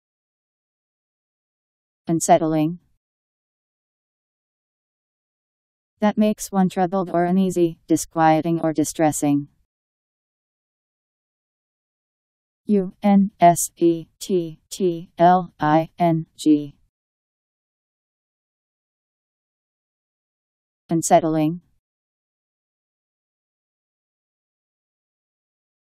Unsettling Meaning
Video shows what unsettling means. That makes one troubled or uneasy; disquieting or distressing..  Unsettling Meaning. How to pronounce, definition audio dictionary. How to say unsettling. Powered by MaryTTS, Wiktionary